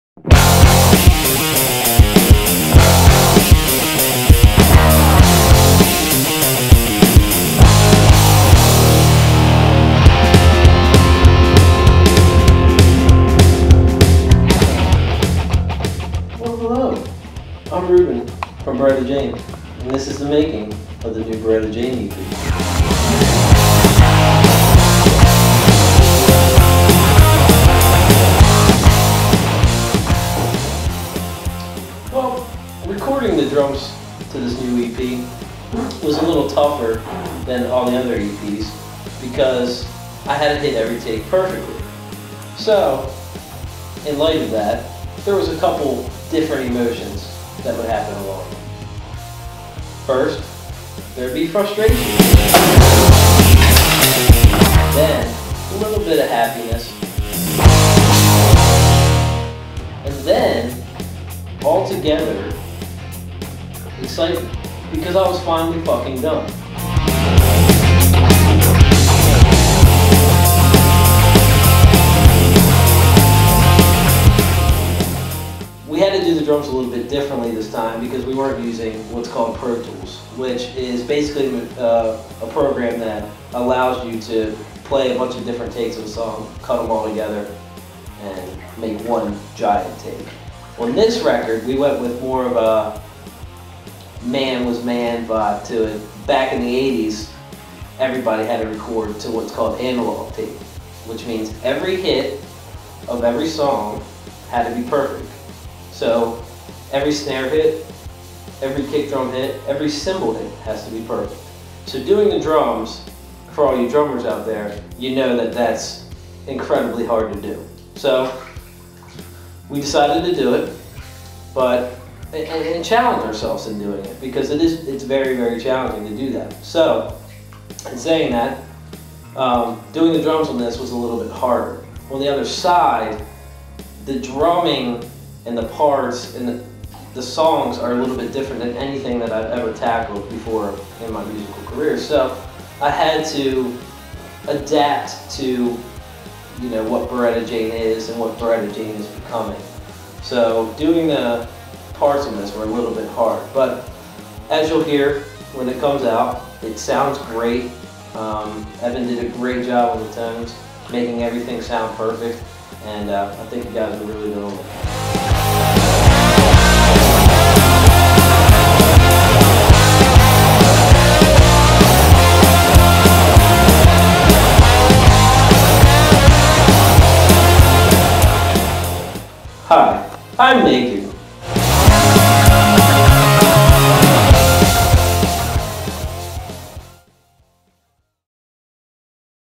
Beretta Jane Video Blog #1
The first installment of the Beretta Jane Video Blogs starts with Rooobyn discussing the NEW! Beretta Jane record and recording all of the drum parts for the new record. Enjoy!